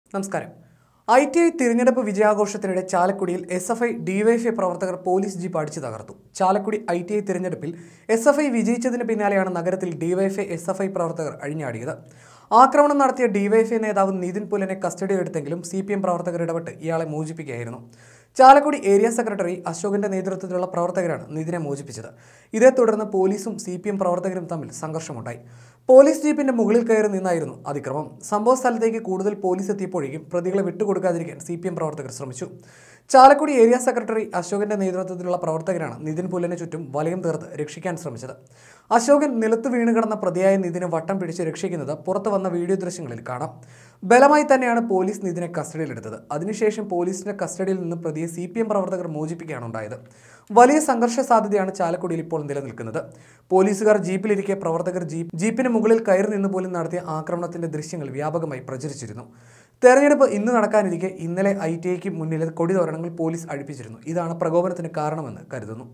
എസ്എഫ്‌ഐ - ഡിവൈഎഫ്‌ഐ പ്രവര്‍ത്തകര്‍ പൊലീസ് ജീപ്പ് അടിച്ചു തകര്‍ത്തു l cpm activists
ഐടിഐ തിരഞ്ഞെടുപ്പ് വിജയാഘോഷത്തിനിടെ ചാലക്കുടിയിൽ എസ്എഫ്ഐ – ഡിവൈഎഫ്ഐ പ്രവർത്തകർ പൊലീസ് ജീപ്പ് അടിച്ചു തകർത്തു. ചാലക്കുടി ഐടിഐ തിരഞ്ഞെടുപ്പിൽ എസ്എഫ്ഐ വിജയിച്ചതിന് പിന്നാലെയാണ് നഗരത്തിൽ ഡിവൈഎഫ്ഐ-എസ്എഫ്ഐ പ്രവർത്തകർ അഴിഞ്ഞാടിയത്.